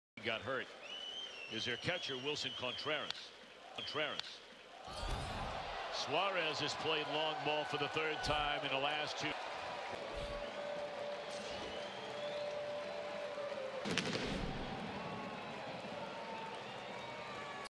Eugenio Suarez clubs a solo home run
Eugenio Suarez clubs a solo home run that lands in the upper-deck seats in left in the bottom of the 9th inning